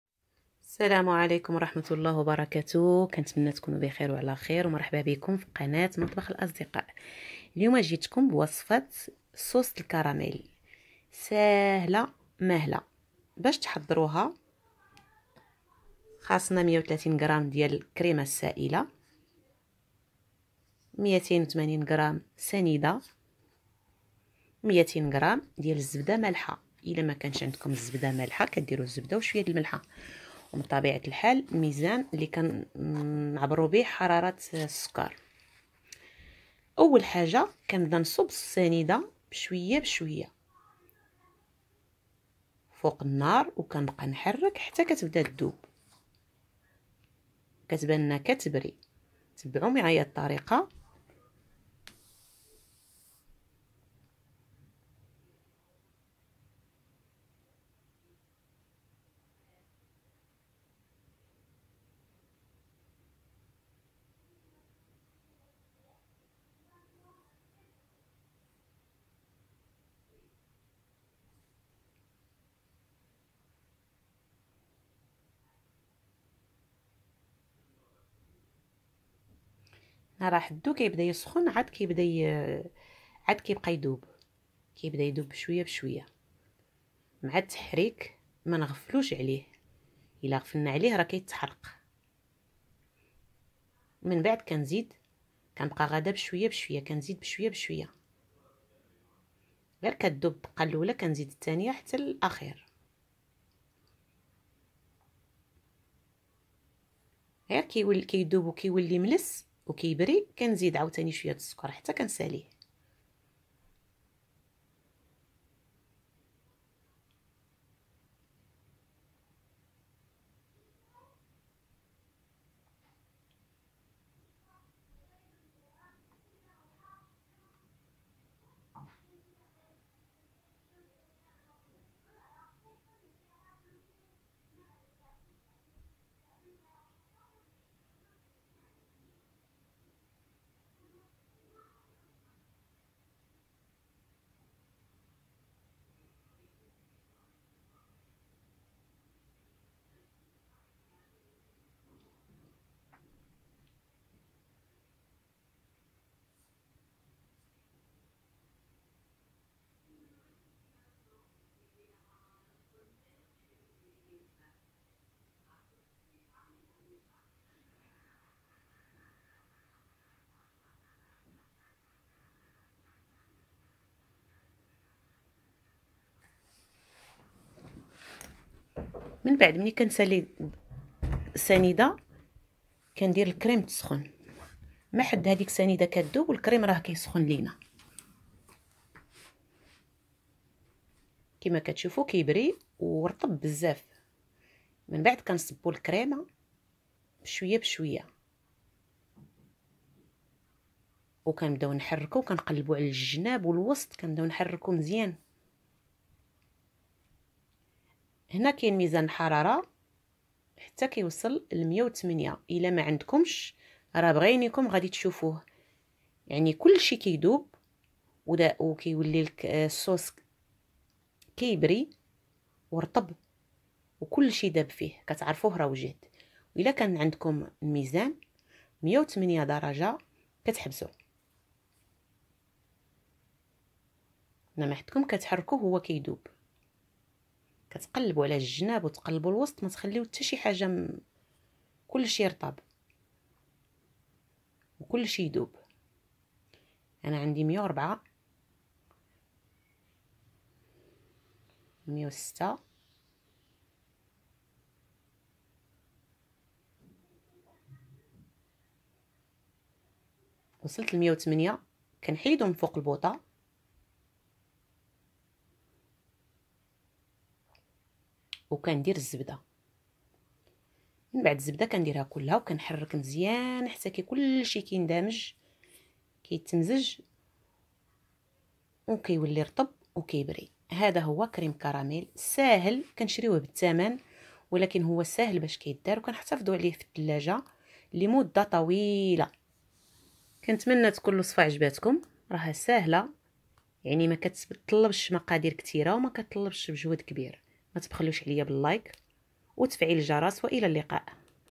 صوص كراميل crème caramel beurre salé
صوص الكراميل ناجح 100%
المقادير
280غ سنيدة
130غ الكريمة السائلة
200غ زبدة مالحة

Crème au beurre salé réussie à 100%

Les ingrédients
280g sucre semoule
130g crème liquide
200g beurre salé